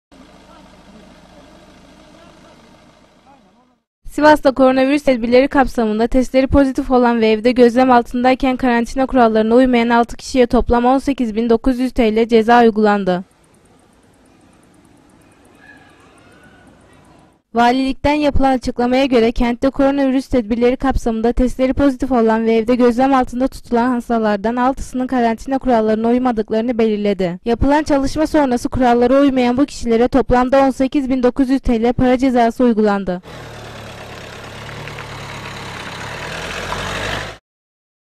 TESTİ POZİTİF ÇIKAN VE KARANTİNAYA UYMAYAN KİŞİLERE CEZA
SİVAS'TA KORONAVİRÜS TEDBİRLERİ KAPSAMINDA TESTLERİ POZİTİF OLAN VE EVDE GÖZLEM ALTINDAYKEN KARANTİNA KURALLARINA UYMAYAN 6 KİŞİYE TOPLAM 18 BİN 900 TL CEZA UYGULANDI